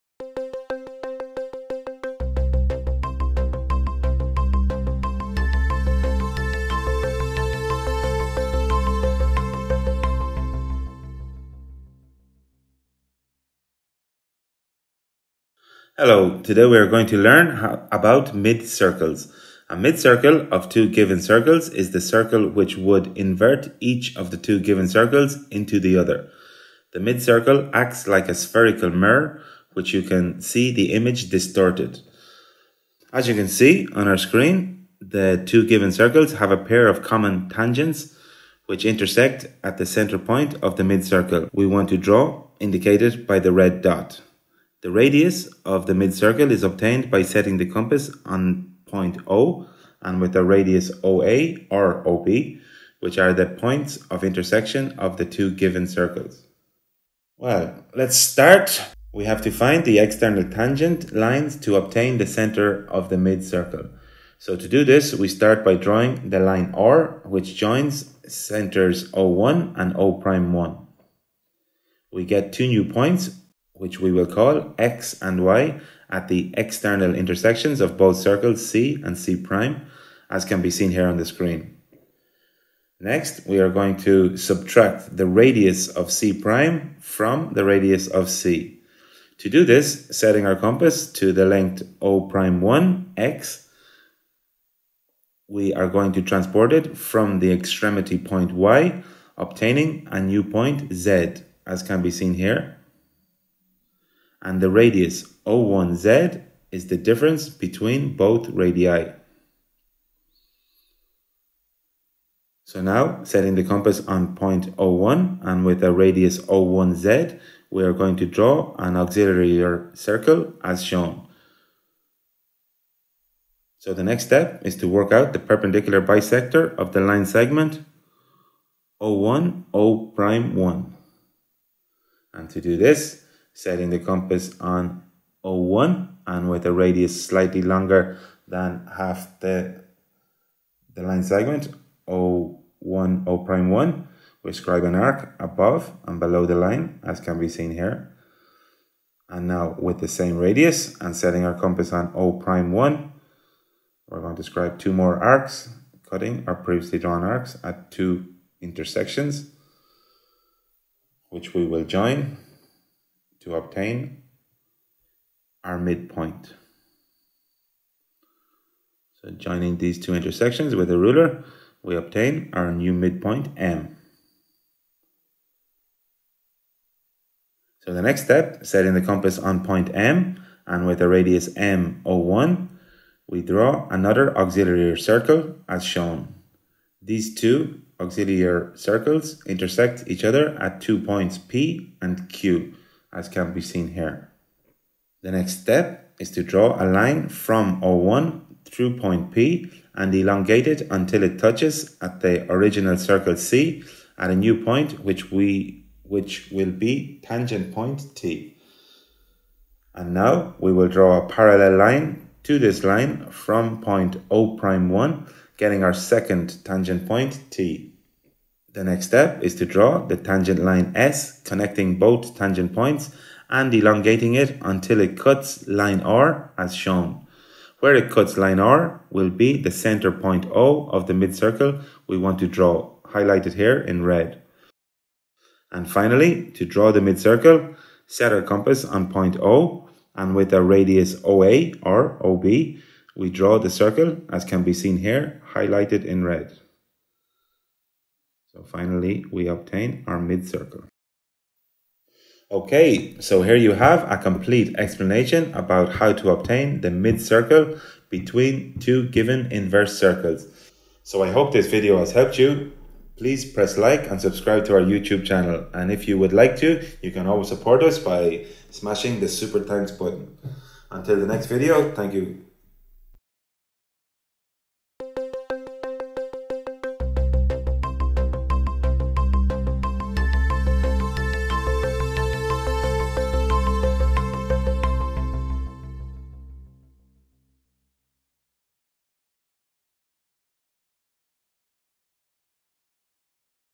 How to Draw the Mid-Circle of two given Circles Intersecting at two points (Inversive Geometry)
Learn how to construct the reference circle of any pair of inverse circles intersecting each other at 2 points.